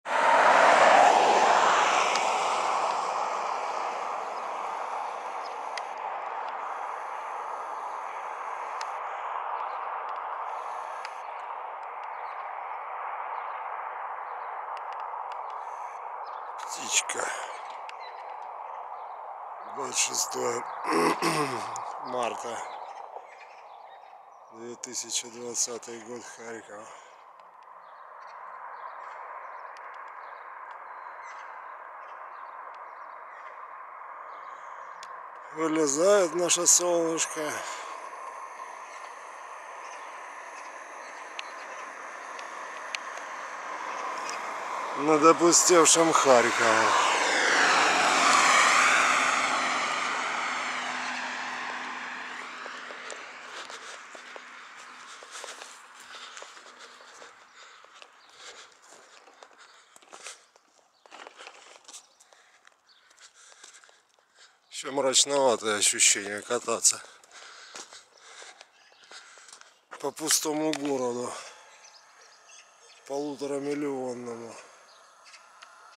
Солнце поднимается над безлюдным Харьковом 26 марта 2020 года
Рассвет в Харькове - Солнце поднимается над горизонтом 26 марта 2020 года, увеличение 50 крат